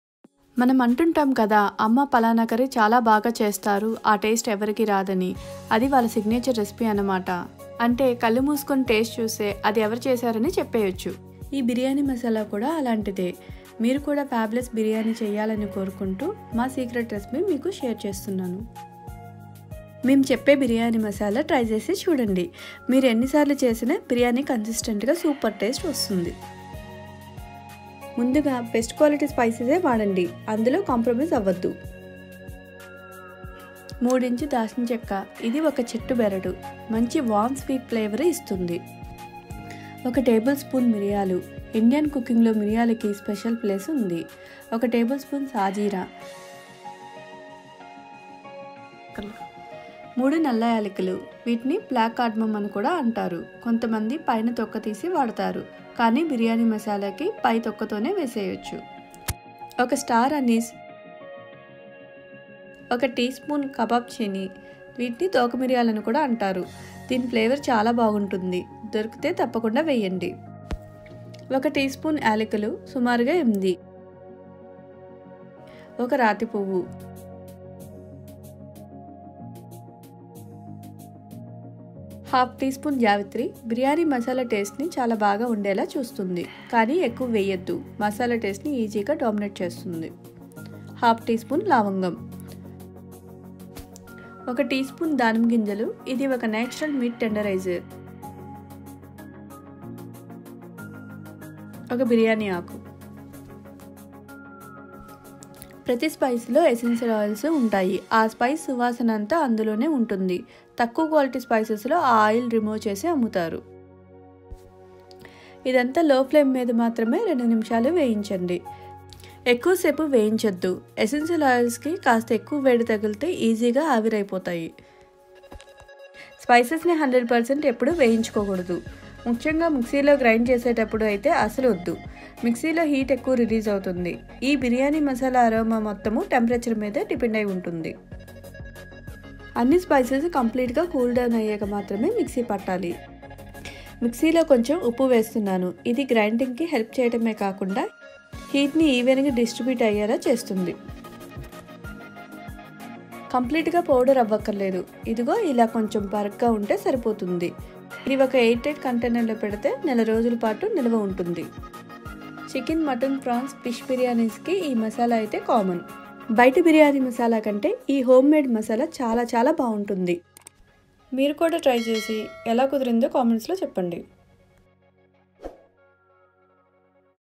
Biryani Masala Recipe | Homemade Biryani Masala Powder | Aromatic Fragrant and Rich
Best home made biryani masala

To make best biryani, biryani masala is the key. This video explains about all the important ingredients to make best home made biryani masala that people are been hunting for years.

By making Biryani masala home made, you get the best flavour of the fresh spices and importantly no preservatives and artificial colouring of store bought ones.

Give a try of our home made biryani masala, you will certainly never go back to store bought one again. We are certain you will like our biryani masala as it is rich with flavours, aromatic and importantly carefully made with love and compassion.

 We can say, you will get same wonderful taste for your biryani each and every time you make one, as the formula for this recipe remains the same for every make.

Enjoy great food!!!